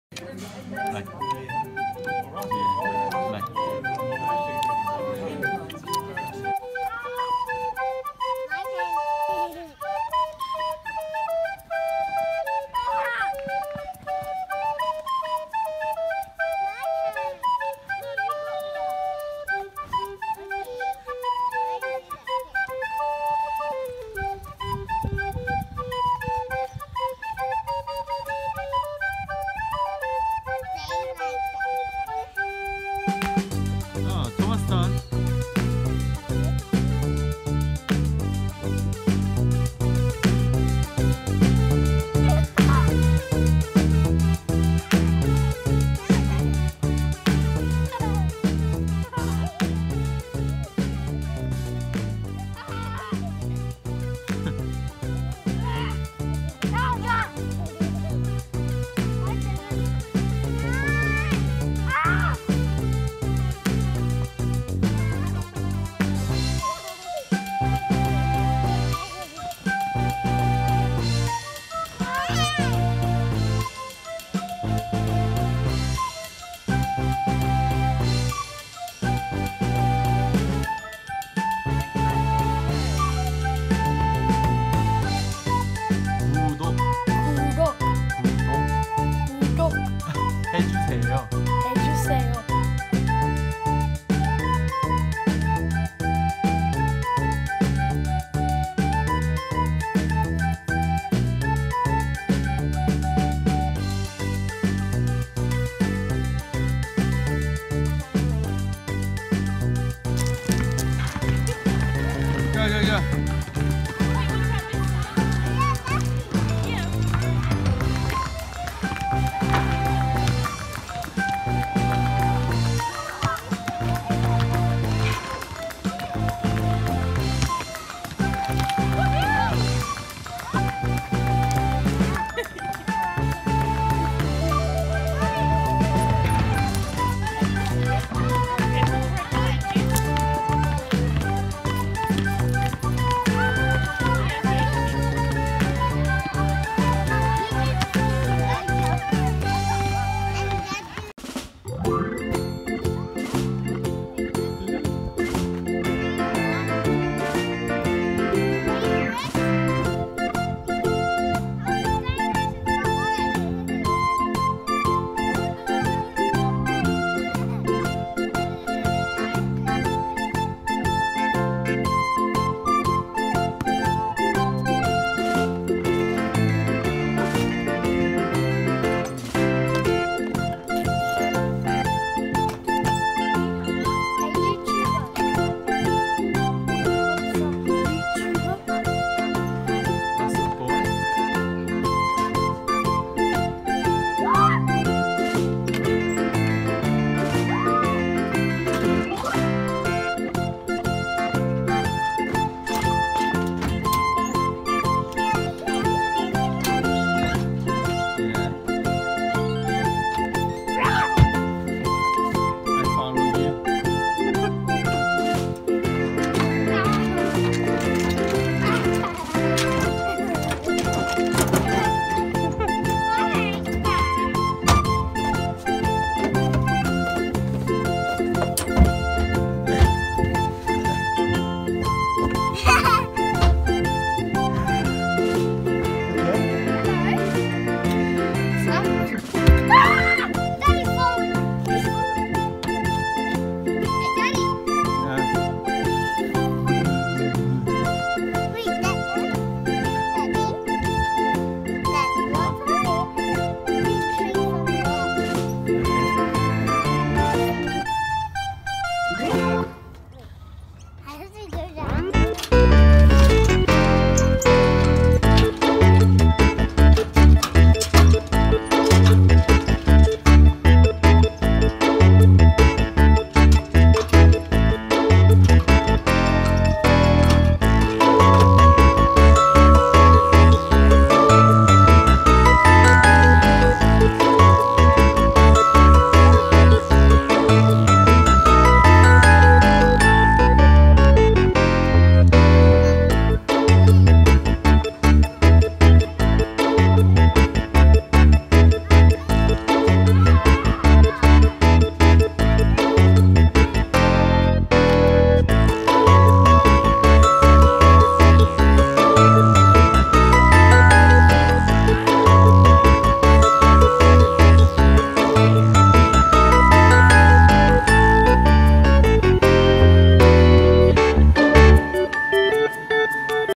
호주 하루살이 장난감 조빙,줄넘기,훌라우프,놀이터 파괴자들
호주 아이들이 노는 생생한 장면들~
심장 약하신분들은 마음의 준비를 하세요.